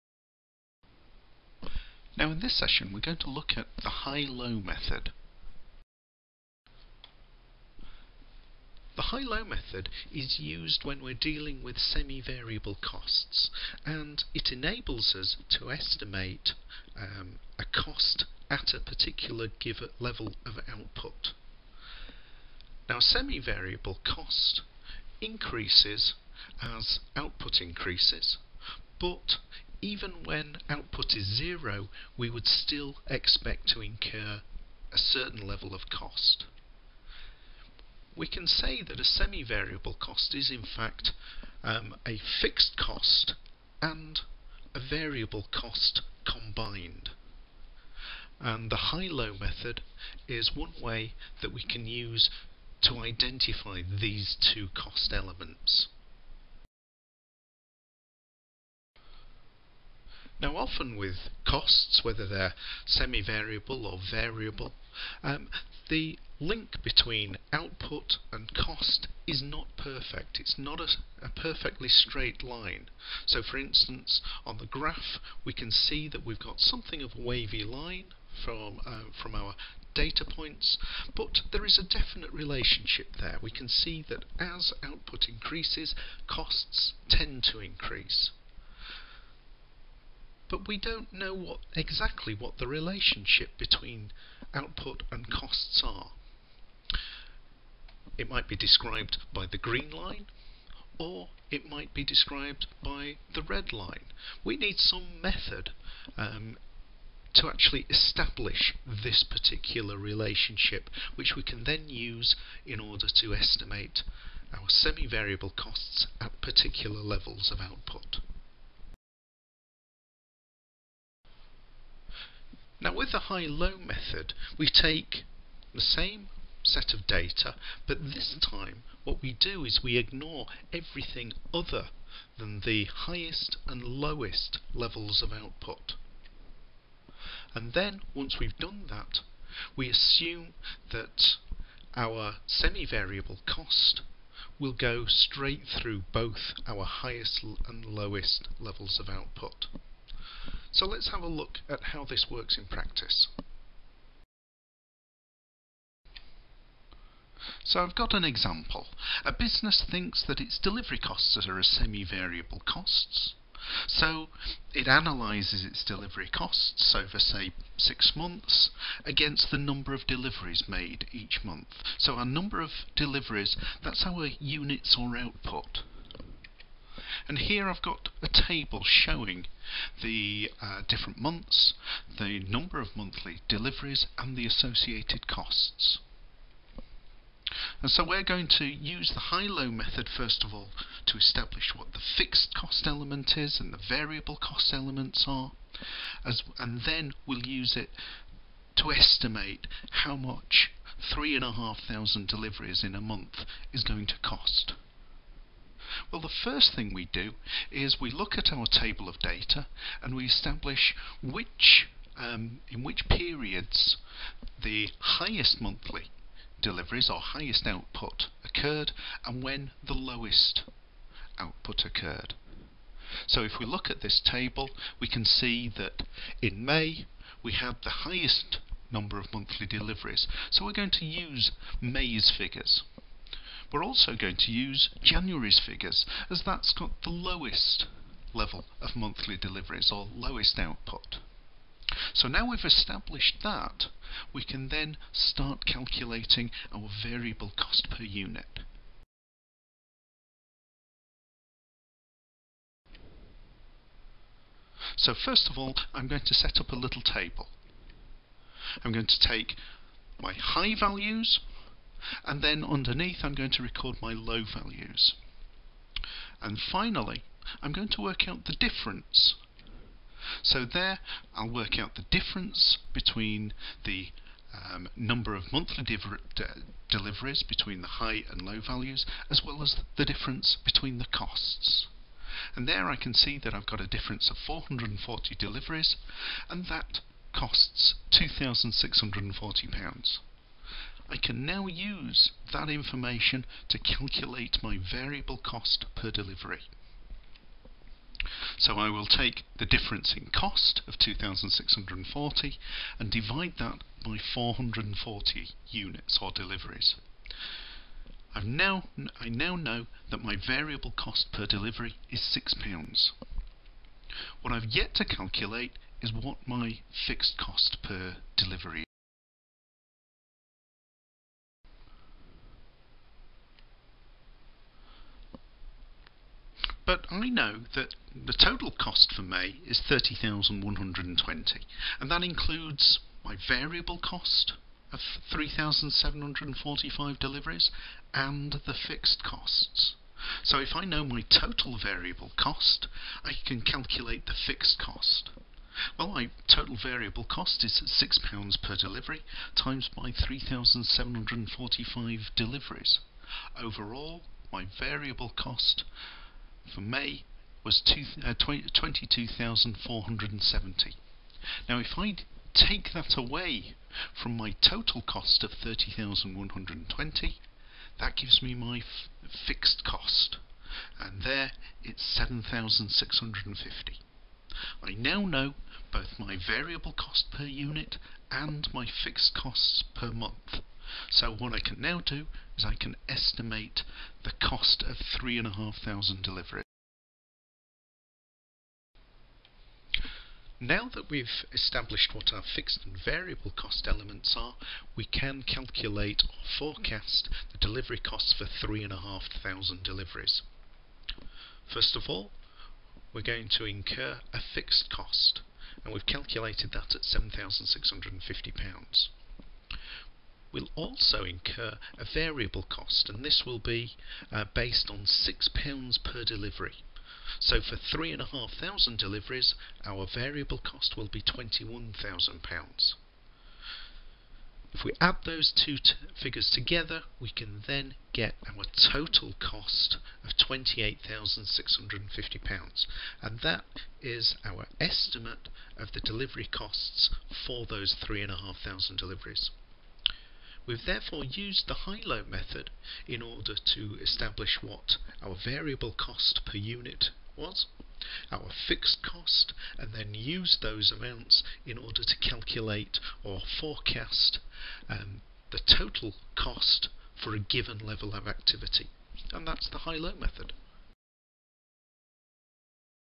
The High Low Method in 7 minutes 40 seconds
A quick explanation and illustration of the high-low method of estimating semi-variable costs at different levels of activity